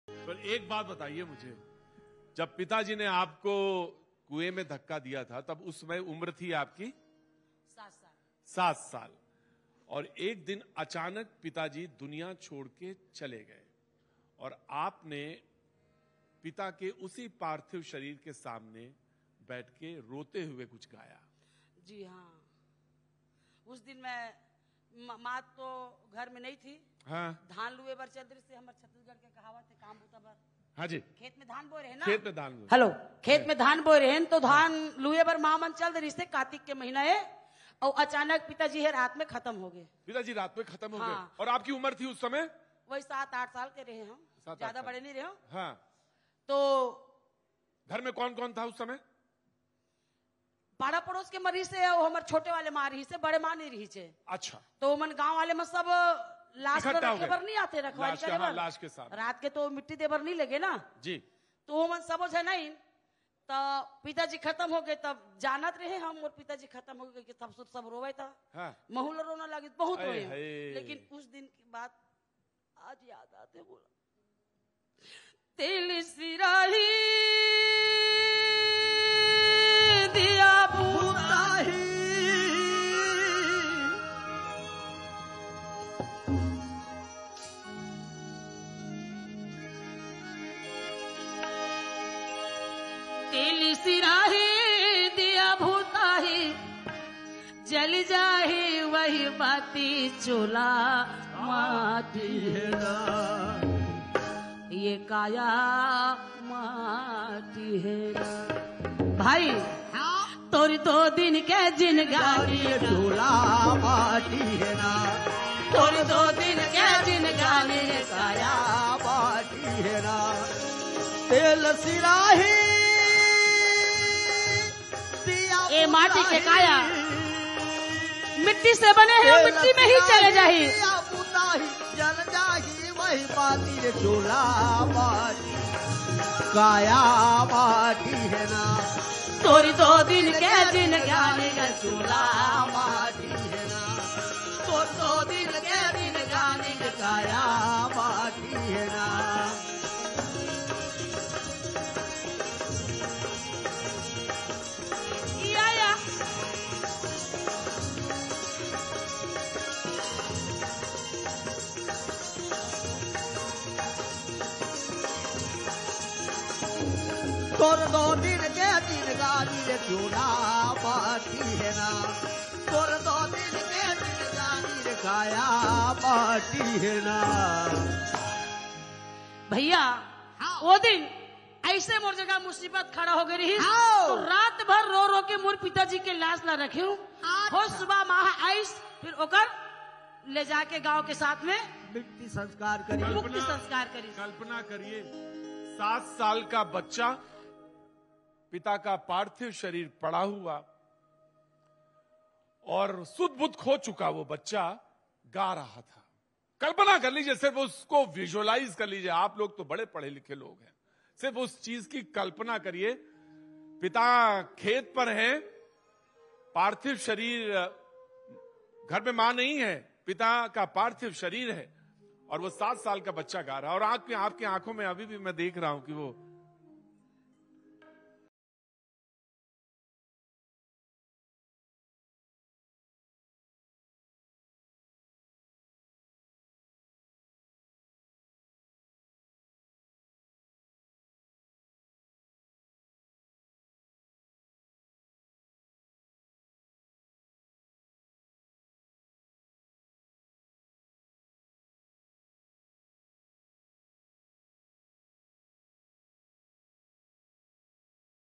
UTSAV18 : Padma Shri Usha Barle ने पिता के पार्थिव शरीर के सामने क्या गुगुनाया था ? | Chattisgarh
Padma Shri Usha Barle ने पिता के पार्थिव शरीर के सामने क्या गुगुनाया था ?